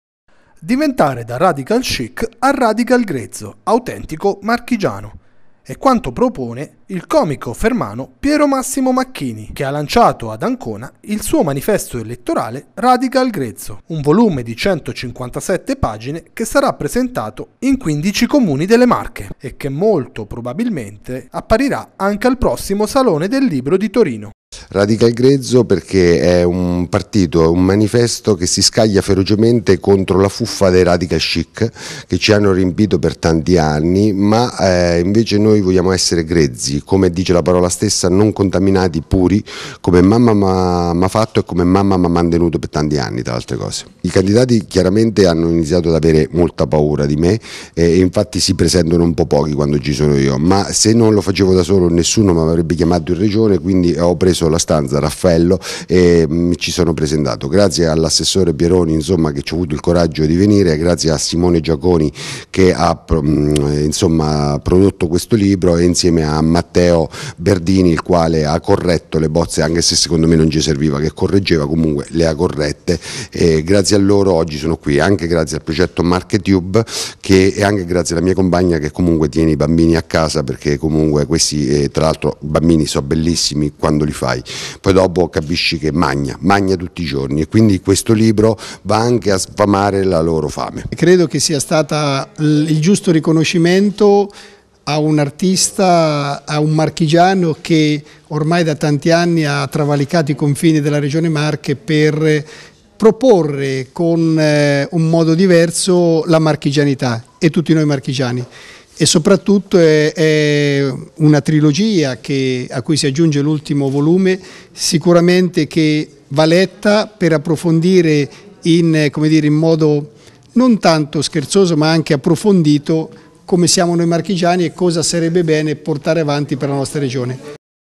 NEWS. Il comico Macchini lancia il manifesto "Radical Grezzo"
"Manifesto Radical Grezzo": un ironico e provocatorio piano di riforme che il comico fermano Piero Massimo Macchini ha presentato ad Ancona. Vediamo...